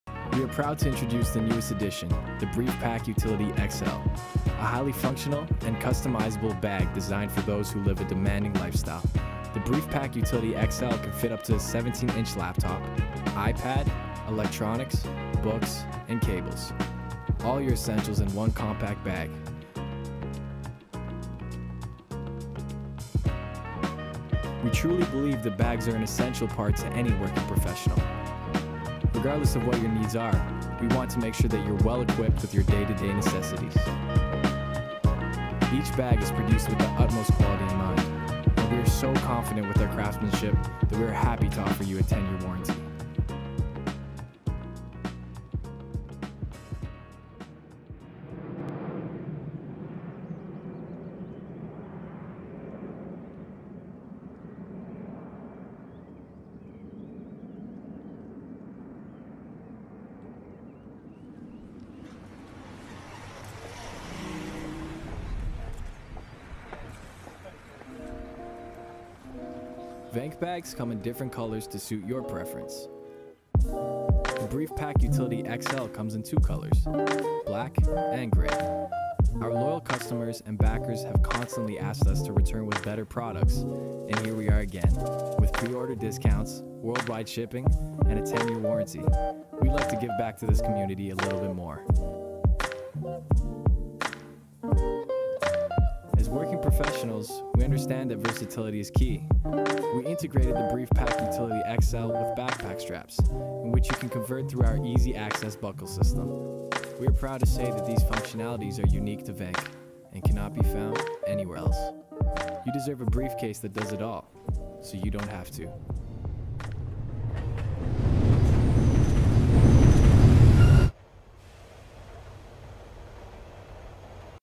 For Busy Professionals: Briefpack XL (Briefcase) - VENQUE
Designed for busy professionals and adventurers alike, the Briefpack was inspired by the classic briefcase, and is built to withstand the demanding needs of the most active travellers. Homepage